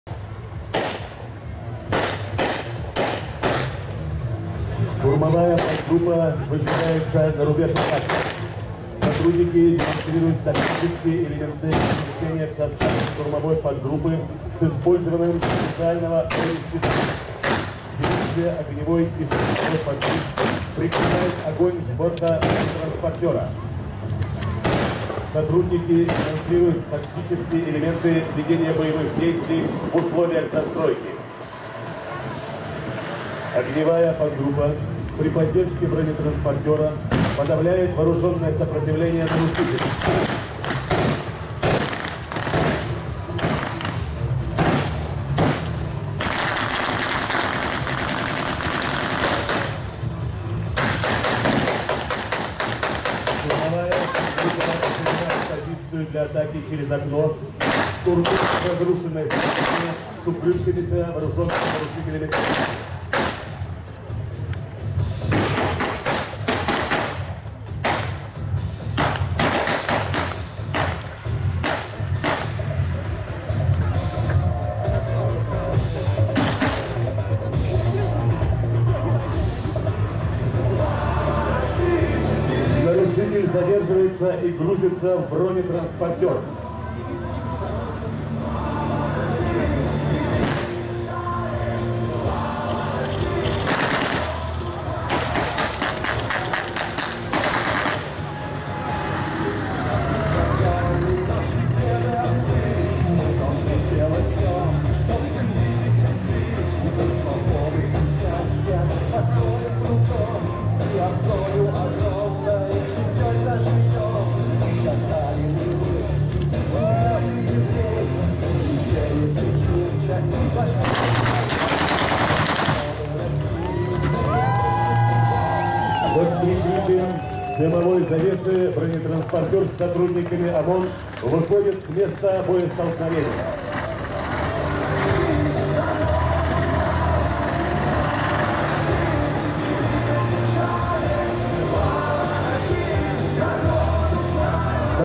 Показательные выступления белгородского ОМОНа
Под Прохоровкой состоялось открытие танкодрома. На празднике, посвященном этому событию, состоялись показательные выступления сотрудников белгородского ОМОНа. Они продемонстрировали ликвидацию пособников бандформирований.